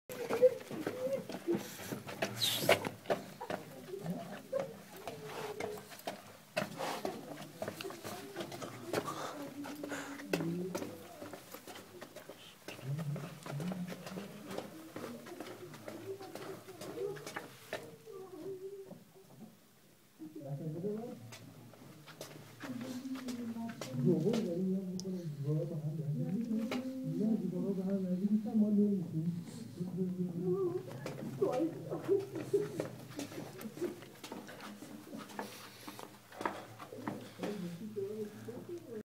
Nooshabad underground city, Iran
There is a historical city named Nooshabad just out of Kashan, Iran where you find a series of tunnels known as the underground city. It was created to protect people from Mongolian invasion of Iran in 13th century!